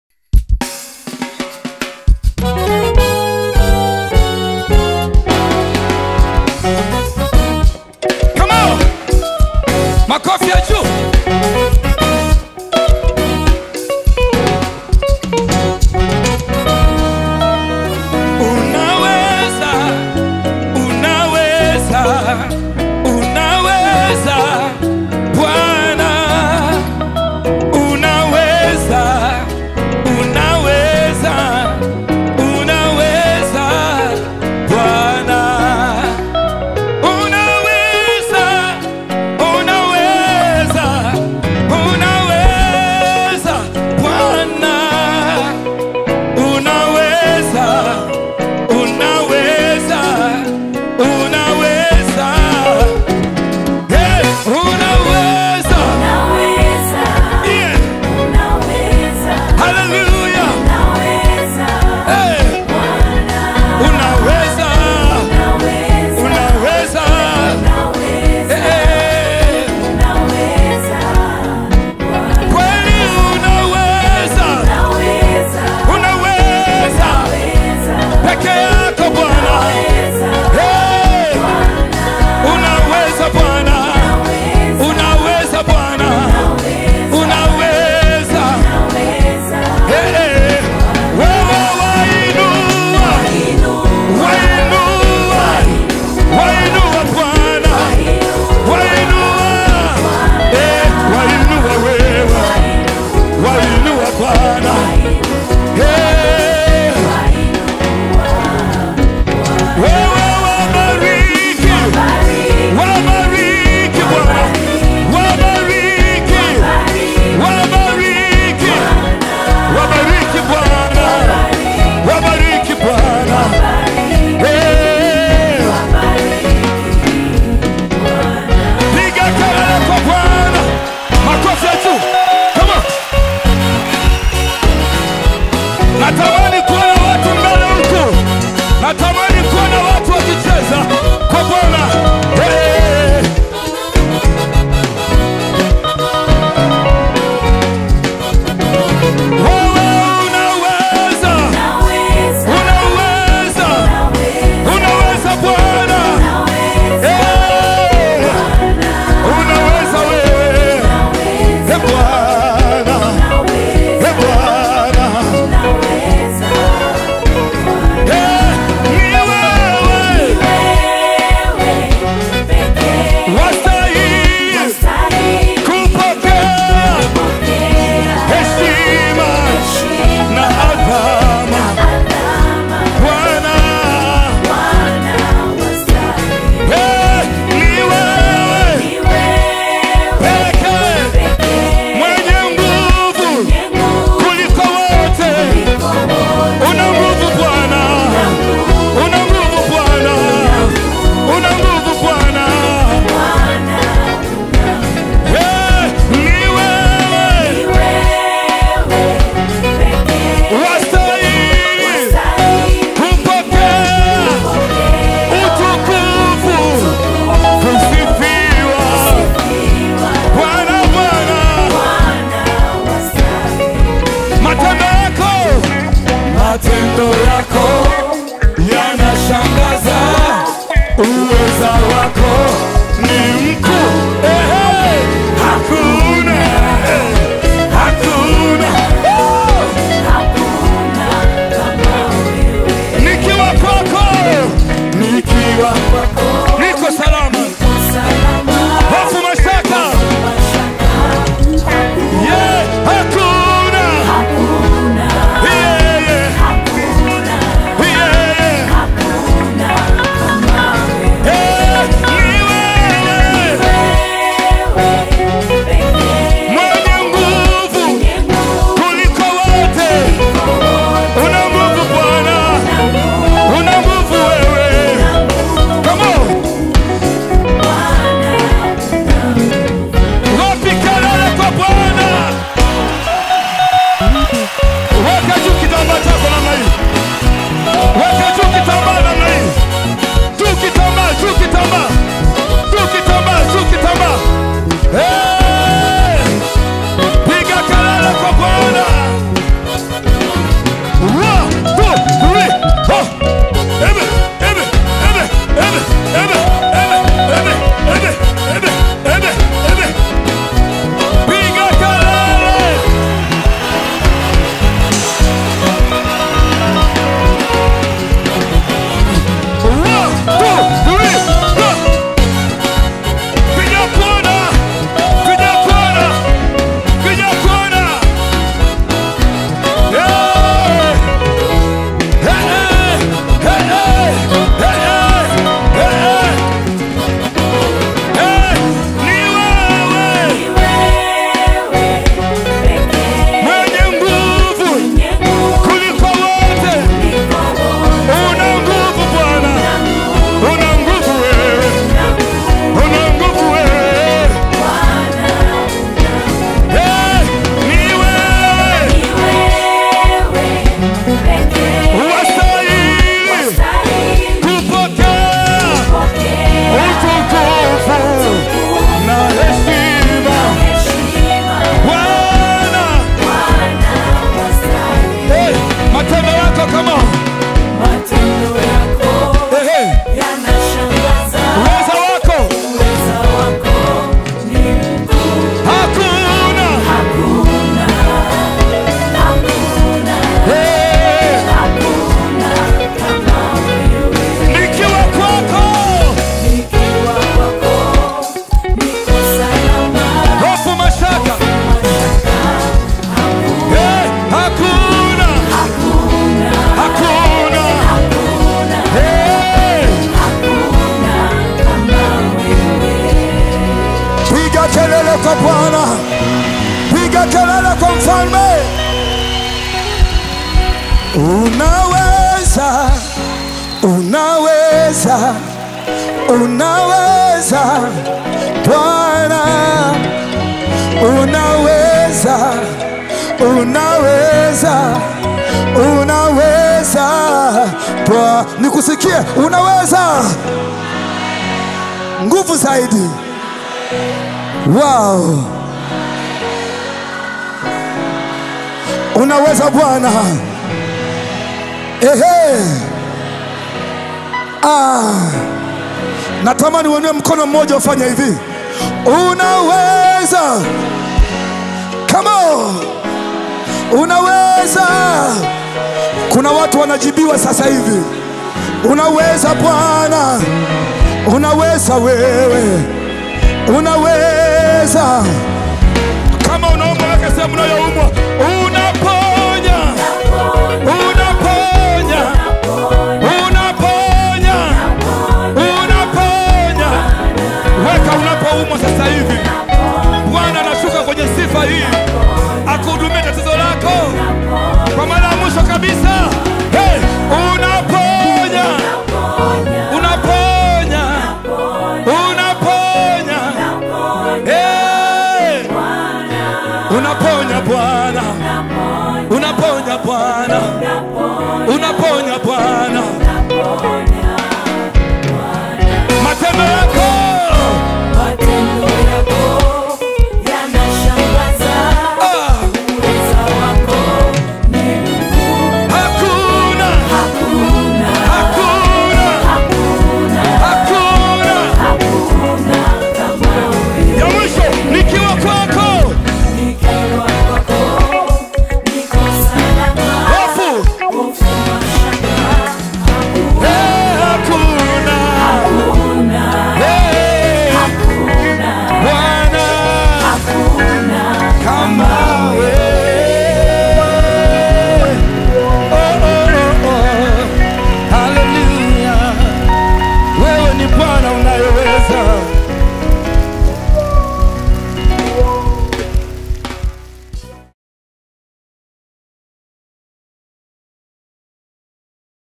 Joseph Vedasto
I thank GOD for making me reach this point, I wish this song to be your help and healing through this song GOD to get you where you want to go, GOD BLESS YOU SO MUCH. - Music director: Shady Nkingx / Charles Nhundu - Main keyboard: Charles Nhundu - Second keyboard: Zakaria Mabula - Third keyboard: Elia Daudi - Lead guitar: Jacob Mmasa - Bass guitar: Shady Nkingx - Drums Joseph Mkombozi Vocal Arrangements: Joseph Vedasto Back Vocals - Mercy Method - Miriam Siima - Joyce Samwel - Christina Kenneth - Charles Kilagela - Mussa Kagoma Over double - Imani Joseph - Josephine Mtumbe - Samwel Mabinda Mixing & Mastering: Shady Nkingx Video editor: Lovebeats